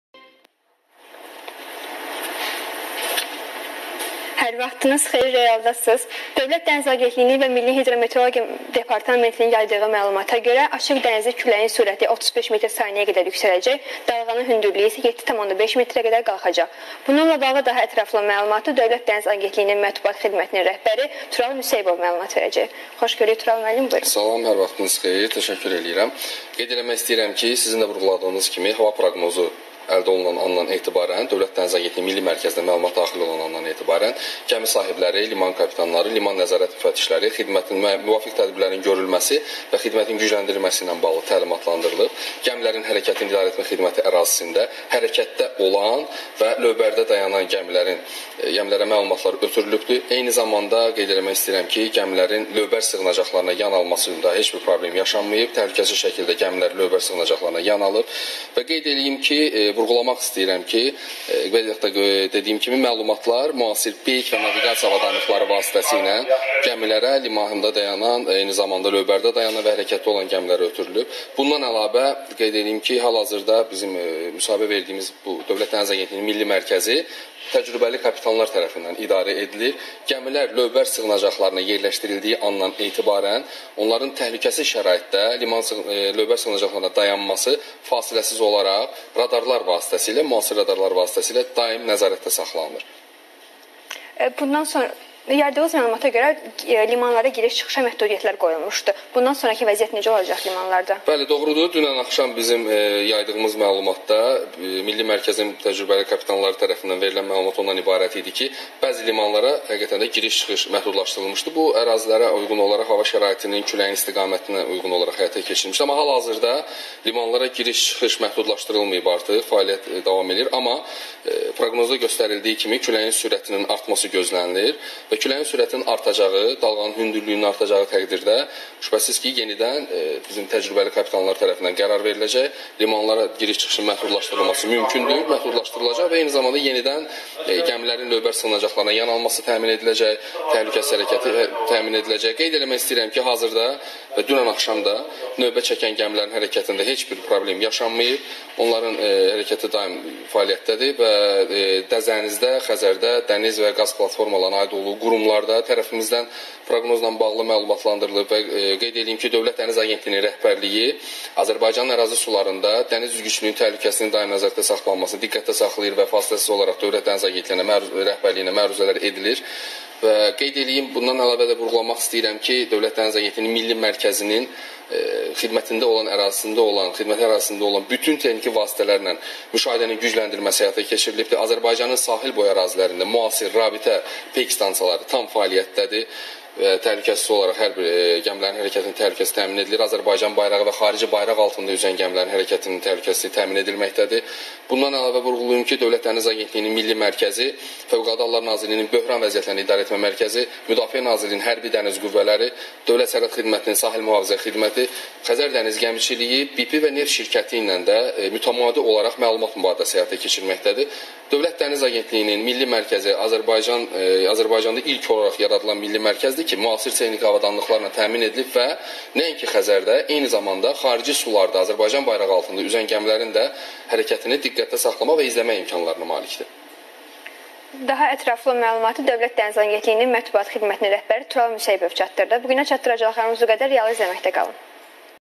Dalğalı Xəzərdə gəmilərin hərəkətinin təhlükəsizliyi
Dövlət Dəniz Agentliyinin Mətbuat Xidmətinin rəhbəri Tural Müseyibov
Real Tv Canlı efir